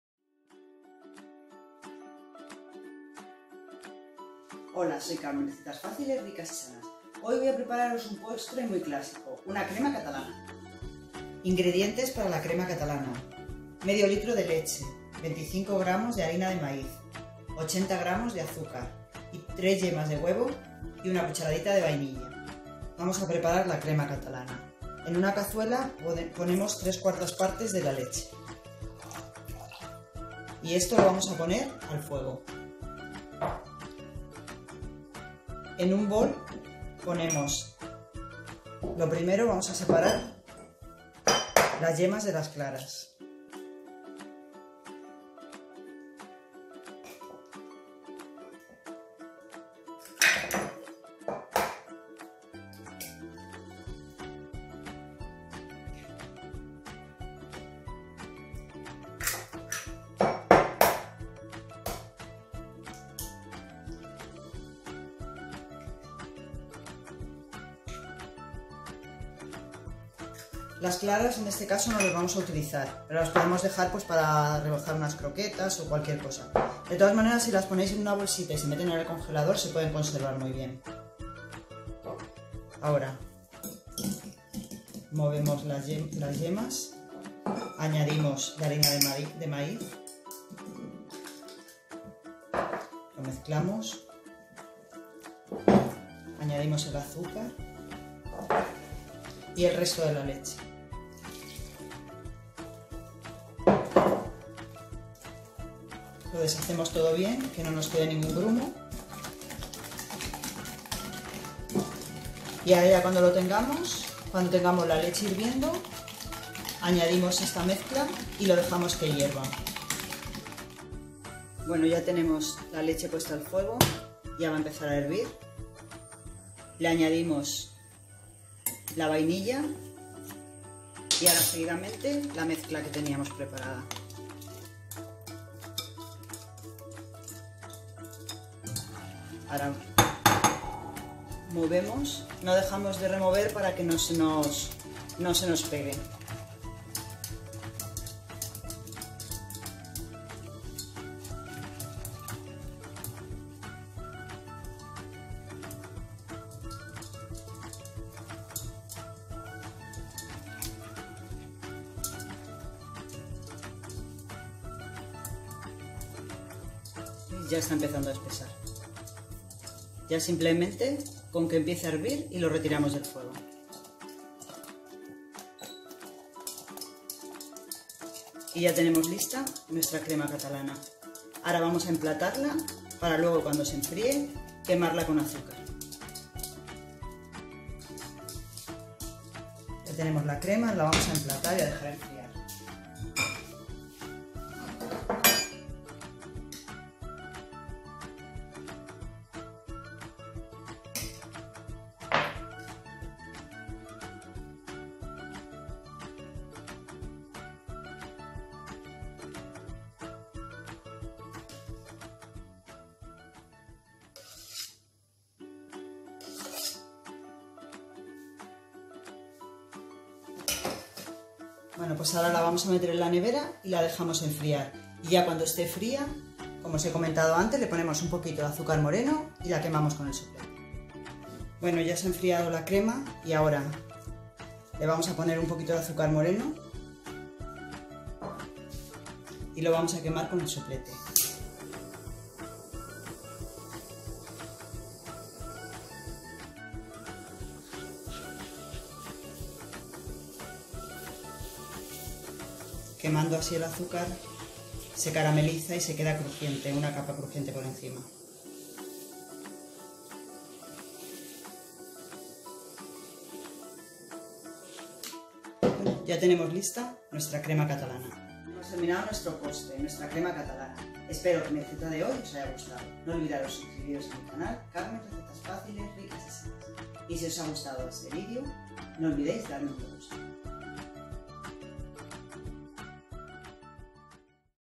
Crema catalana, receta clasica prueba esta receta, te gustara es fácil y rica...
Ingredientes:
500gr de leche
25gr de harina de maíz
80gr de azúcar
3 yemas de huevo
1 cucharadita de vainilla
azucar moreno para quemar